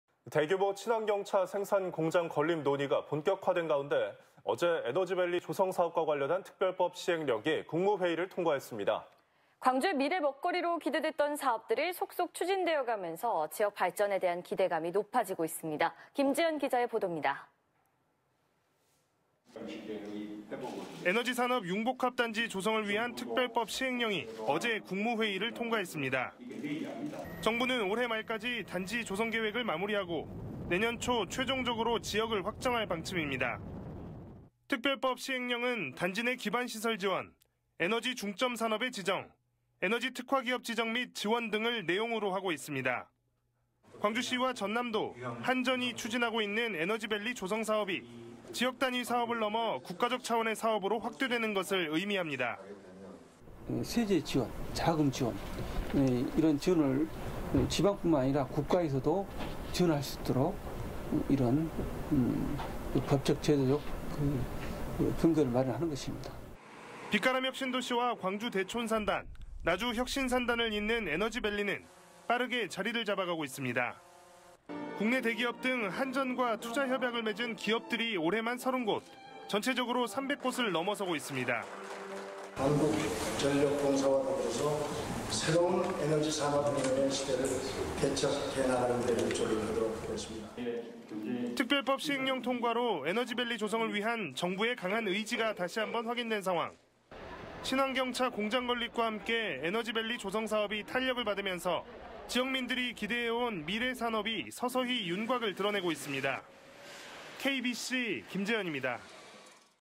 친환경차 이어 에너지신산업 메카 도약
대규모 친환경차 생산 공장 건립 논의가 본격화된 가운데 어제(5일) 에너지밸리 조성 사업과 관련한 특별법 시행령이 국무회의를 통과했습니다.

광주의 미래먹거리로 기대됐던 사업들이 속속 추진되어 가면서 지역 발전에 대한 기대감이 높아지고 있습니다.